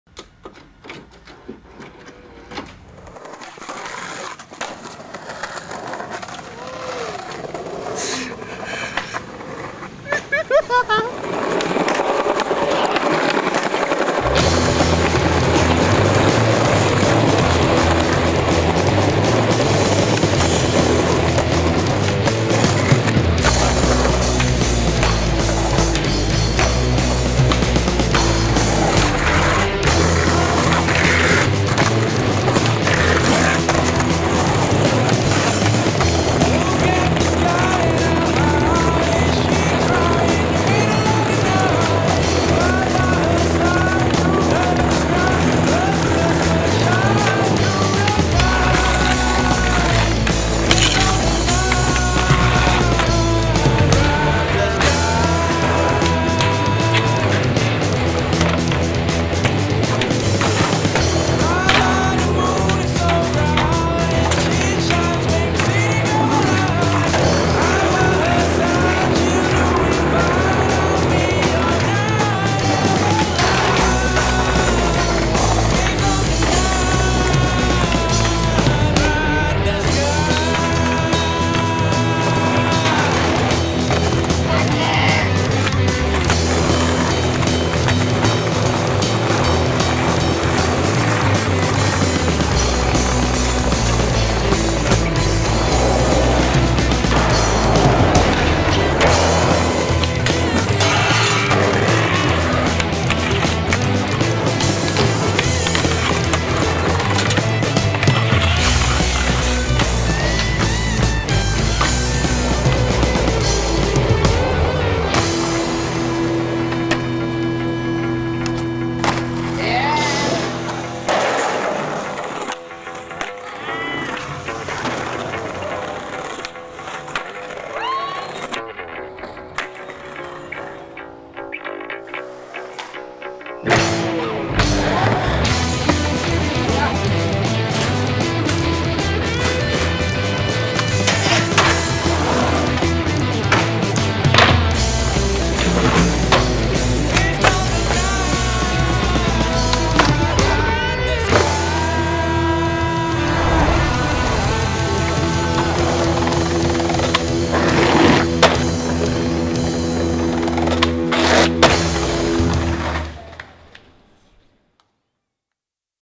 Lost Tapes: Padin Musa
Dedicated to Malaysia. Presenting the most decorated skateboarder some unseen unreleased footages in the spirit of Merdeka! No doubt, Pa'din is a living legend skateboarder in Malaysia. Happy Merdeka! That's independence day for all of you out there.